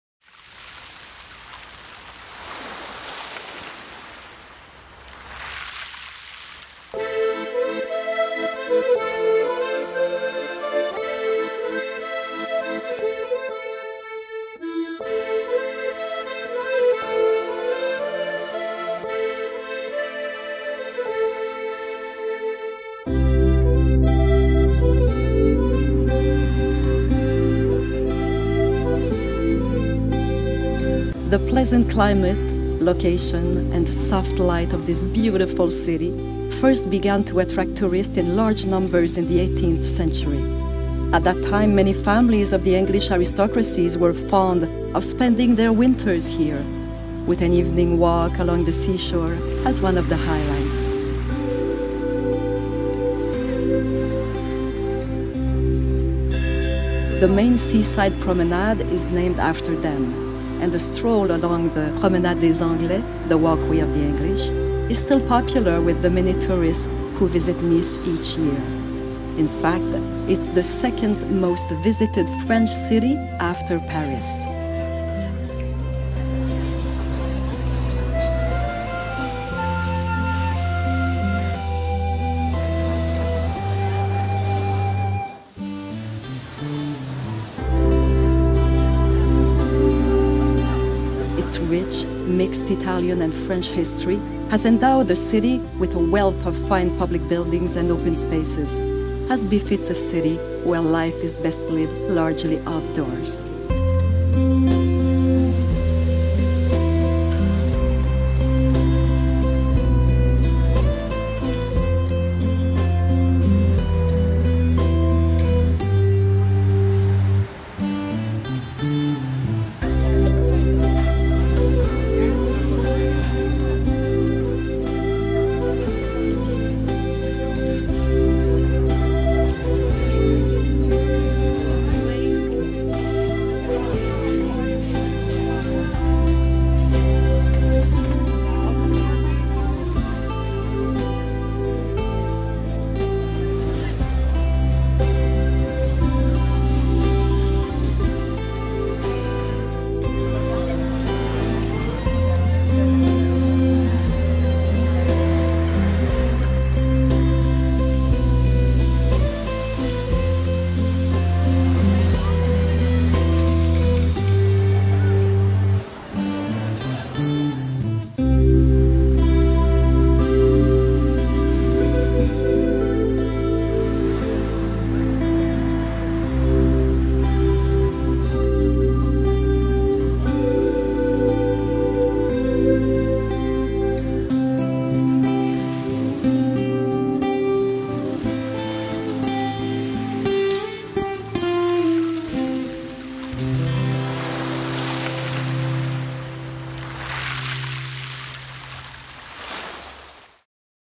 Ницца (Nice, France)
Ни́цца — средиземноморский город и порт на юге Франции. Административный центр департамента Приморские Альпы. Более 352 тысяч жителей. Крупный транспортный узел. Одно из главных курортных мест Лазурного Берега.